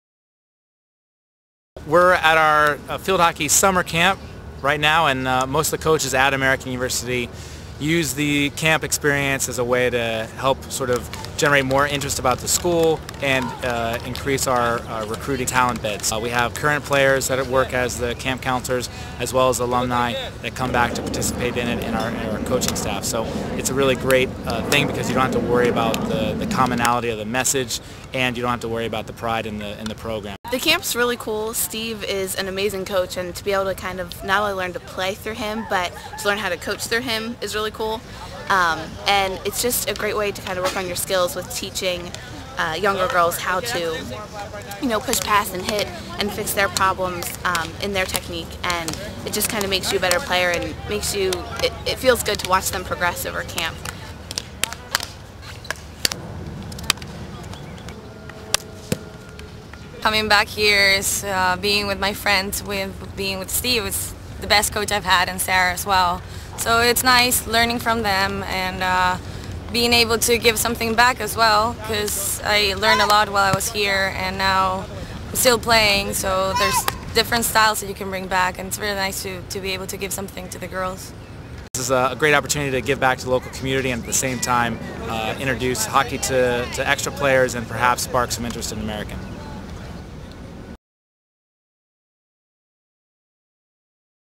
Steve Jennings Field Hockey Camp: American University Athletics
Many of American University's coaches, including field hockey's five-time Patriot League Coach of the Year Steve Jennings, run summer camps at American. Here's an inside look into Jennings' operations, which includes interviews with three-time All-American Javiera Villagra '05 and two-time All-Patriot League selection Savannah Graybill. for more info on American University Athletics.